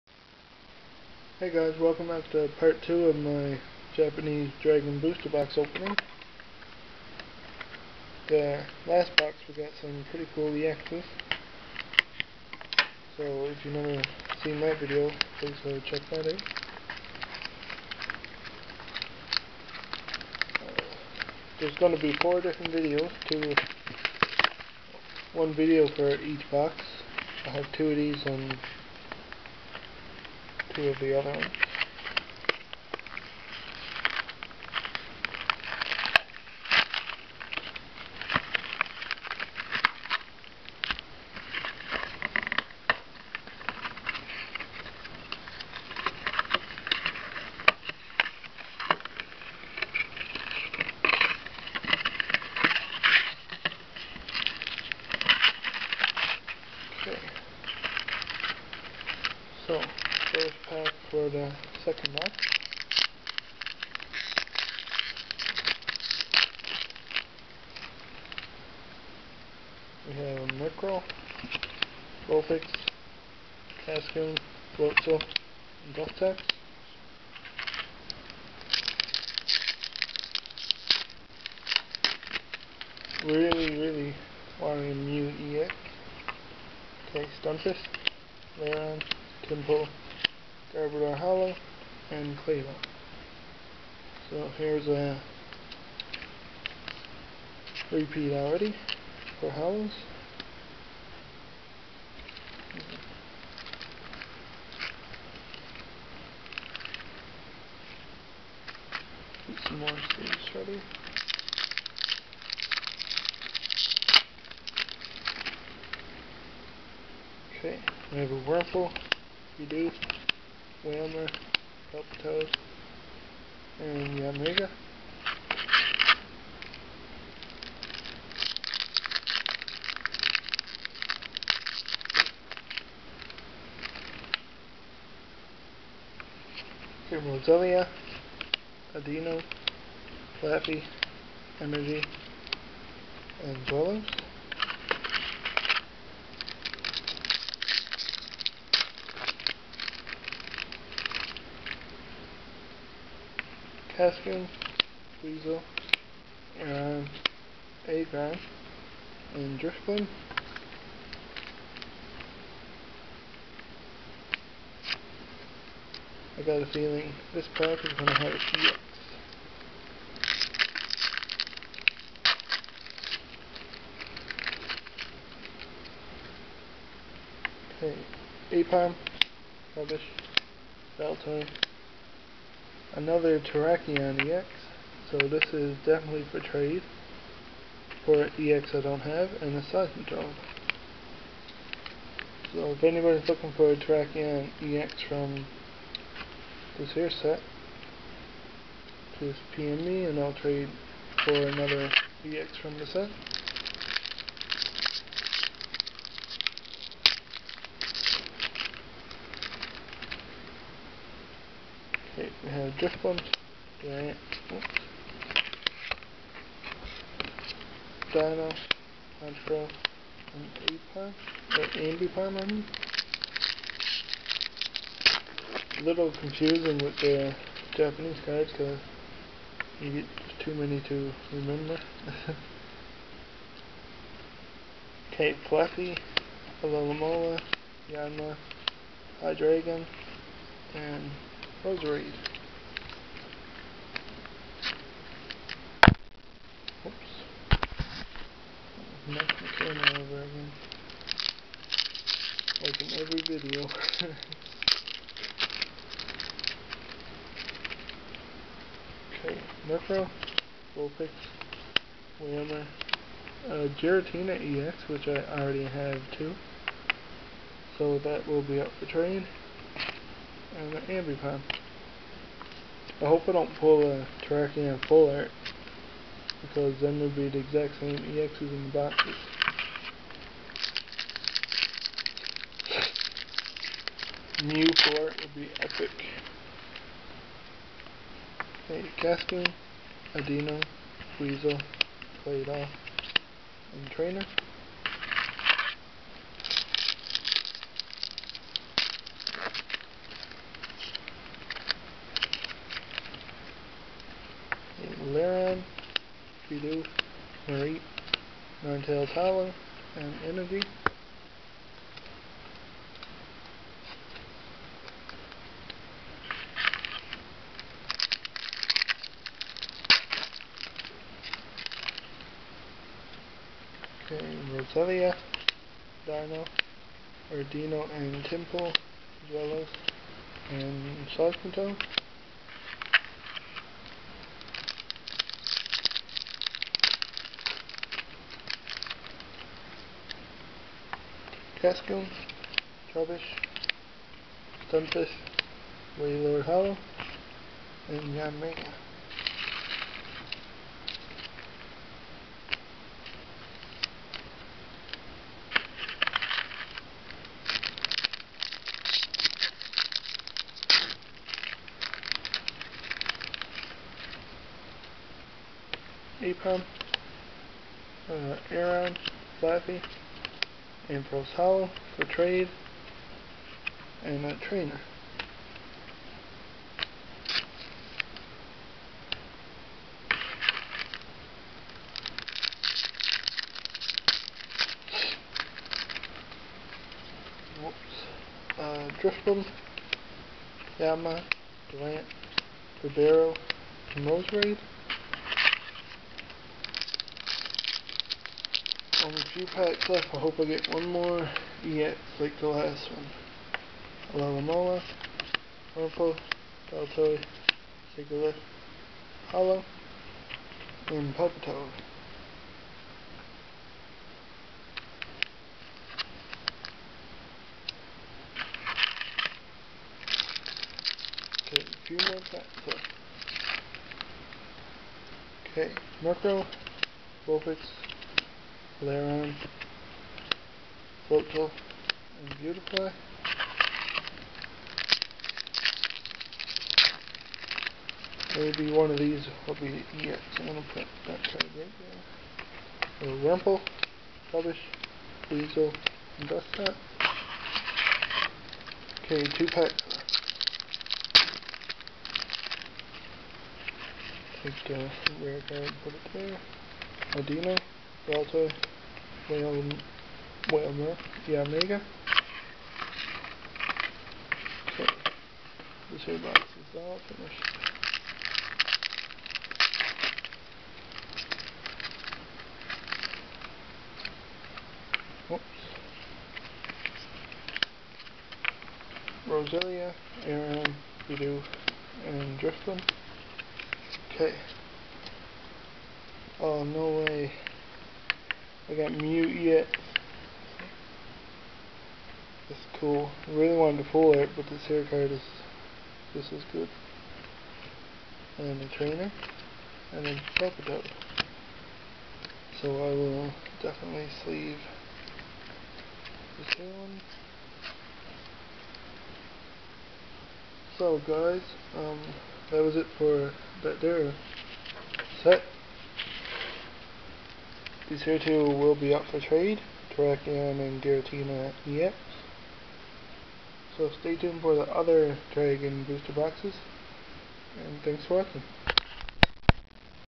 Japanese Dragon Blast Booster Box Opening Box #2(Cool EX!)
Hey guys, I got a few repeats in this box we they will be up for trade for any ex I dont have. So freaken happy I pulled this ex :)